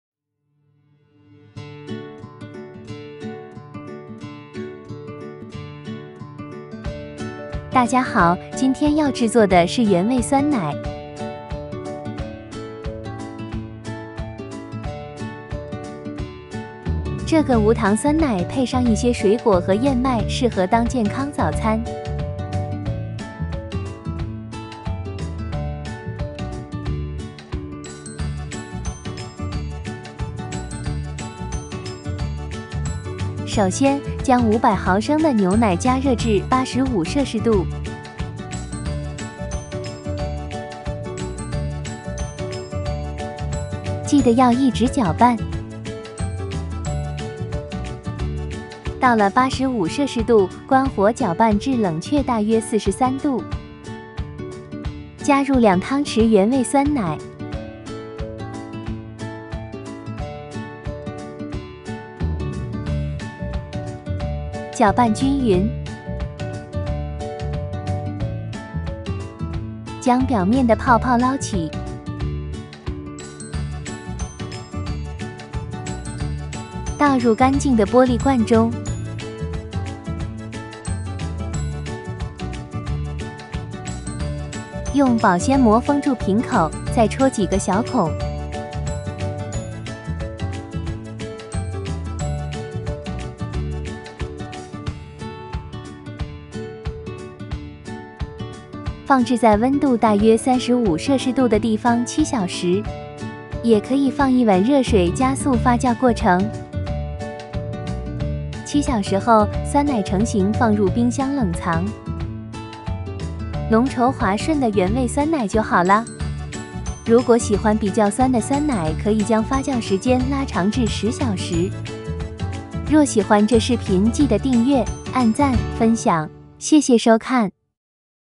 DIY plain yogurt 自制酸奶
Simple and easy DIY yogurt. Save money and healthy choice for breakfast.